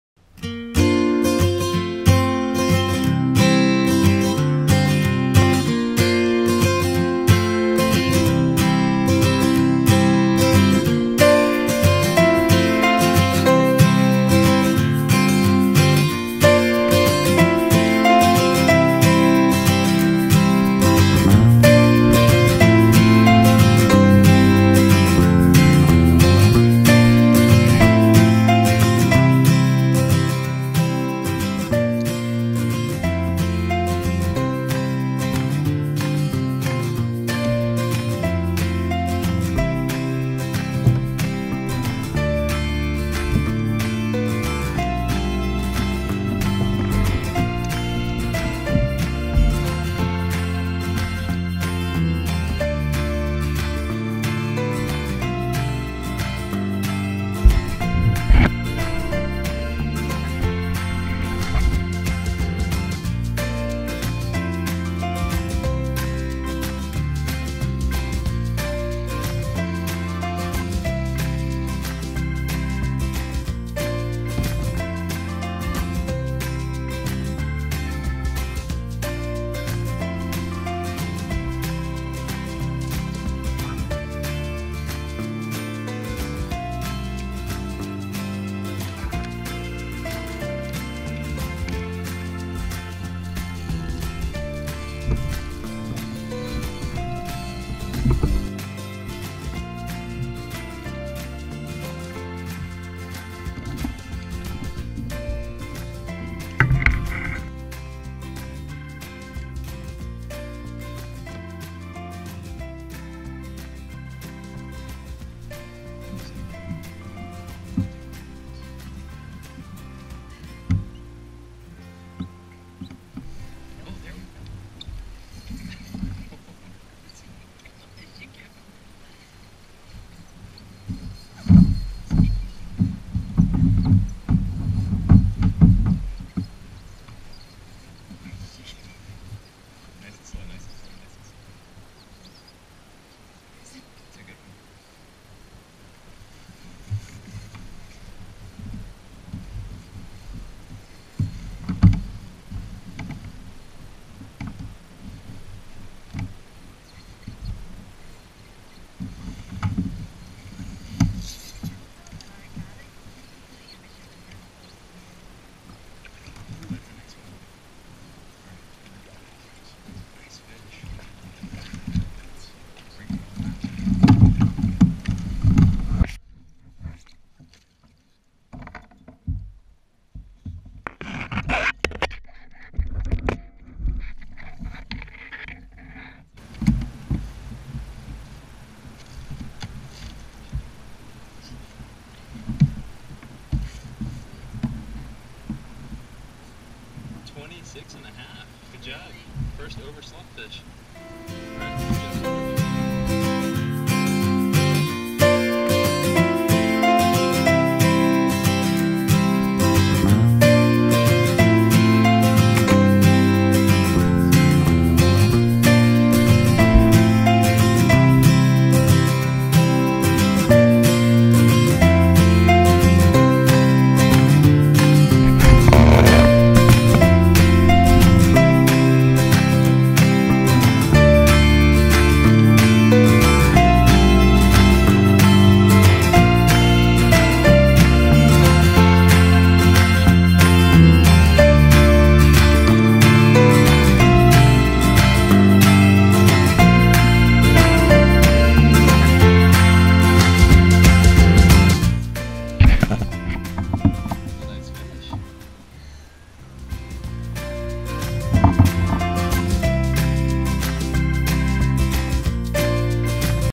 Overslot Red Drum Kayak Fishing
Report: The same rig as previously mentioned was used with head-on shrimp (60/80 ct). The reds were finicky for a long time, only biting the heads of the shrimp off.  We adjusted the hook location and started catching more. The back creeks warm up quicker than the main waterways and reds can be found in these areas under specific conditions, but are easily spooked. Standing up on the kayak to see the fish from above is my favorite way to scout an area. Her largest fish went 26.5", which was her first Red fish and first over slot fish.